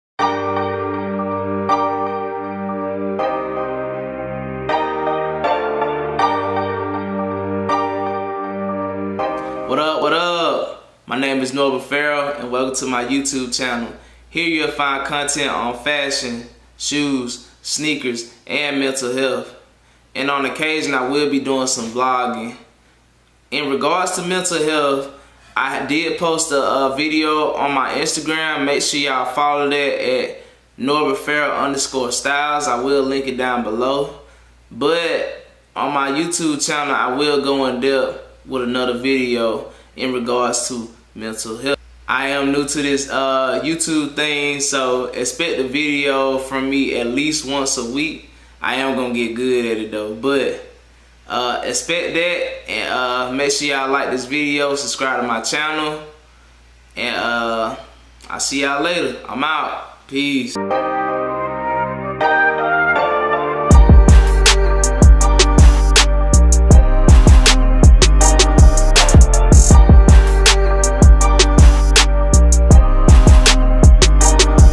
My First Youtube Video!
Waddup My name is Norbert Ferrell and this is my first Youtube video. It might not be the best but i will get better.

I will be talking about fashion, sneakers and mental health